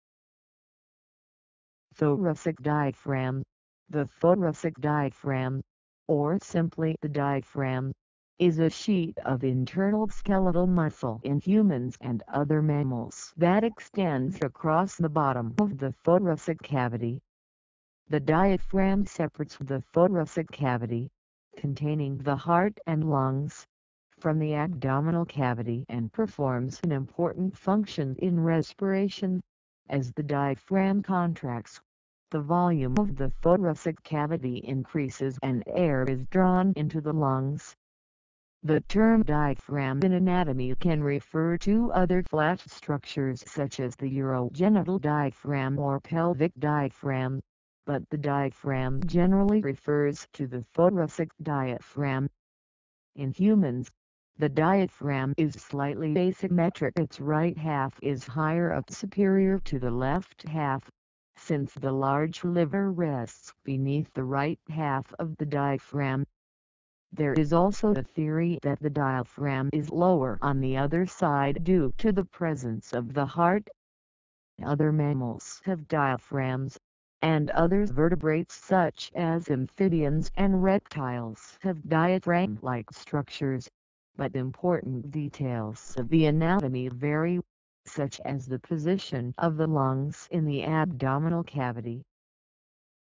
What Is THORACIC DIAPHRAGM? THORACIC DIAPHRAGM Definition & Meaning
What is THORACIC DIAPHRAGM, What does THORACIC DIAPHRAGM mean, THORACIC DIAPHRAGM meaning, THORACIC DIAPHRAGM definition, THORACIC DIAPHRAGM  explanation

The thoracic diaphragm, or simply the diaphragm (Ancient Greek: διάφραγμα, translit. diáphragma, lit. 'partition'), is a sheet of internal skeletal muscle[2] in humans and other mammals that extends across the bottom of the thoracic cavity. The diaphragm separates the thoracic cavity, containing the heart and lungs, from the abdominal cavity and performs an important function in respiration: as the diaphragm contracts, the volume of the thoracic cavity increases and air is drawn into the lungs.

The term diaphragm in anatomy can refer to other flat structures such as the urogenital diaphragm or pelvic diaphragm, but "the diaphragm" generally refers to the thoracic diaphragm. In humans, the diaphragm is slightly asymmetric—its right half is higher up (superior) to the left half, since the large liver rests beneath the right half of the diaphragm. There is also a theory that the diaphragm is lower on the other side due to the presence of the heart.

Other mammals have diaphragms, and other vertebrates such as amphibians and reptiles have diaphragm-like structures, but important details of the anatomy vary, such as the position of the lungs in the abdominal cavity.
Source: Wikipedia.org